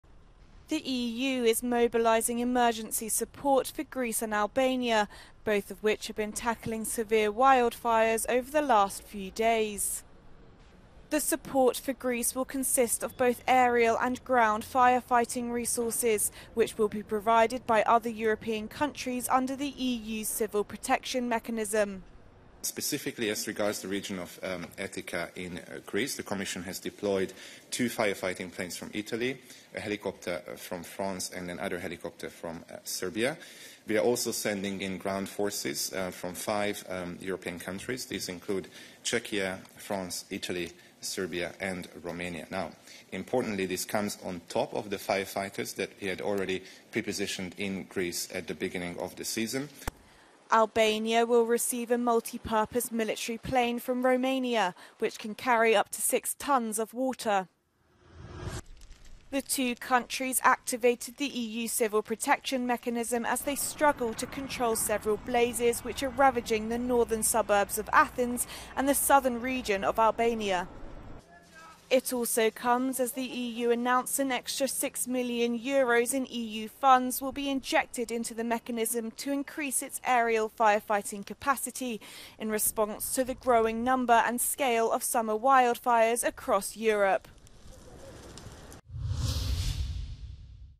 EU to provide emergency support to help Greece and Albania tackle wildfires
The support for Greece will consist of both aerial and ground firefighting resources, Albania will receive a multipurpose military plane from Romania, which can carry up to 6 tonnes of water.